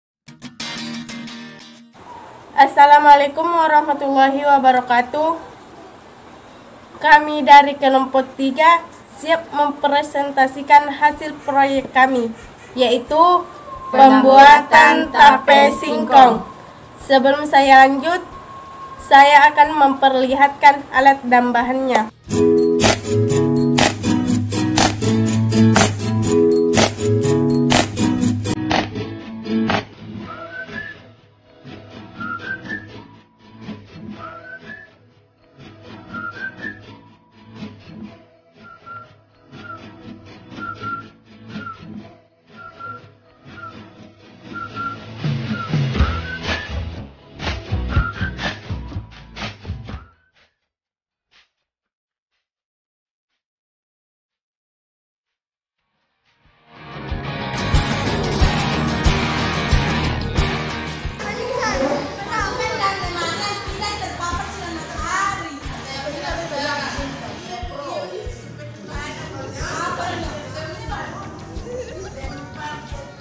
Penerapan Bioteknologi Pangan "Pembuatan Tapai Singkong"
Presentasi peserta didik MTs. Babussalam Kalukuang sebagai salah satu tugas praktikum ujian sekolah kelas IX mata oelajaran IPA Terpadu.